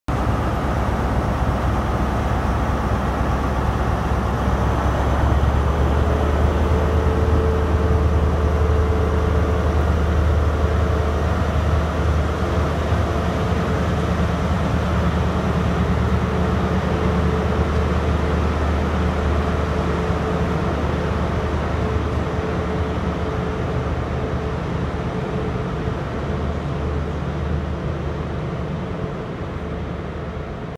43174 at Exeter st Davids 2018
Going to London Paddington on 7.4.18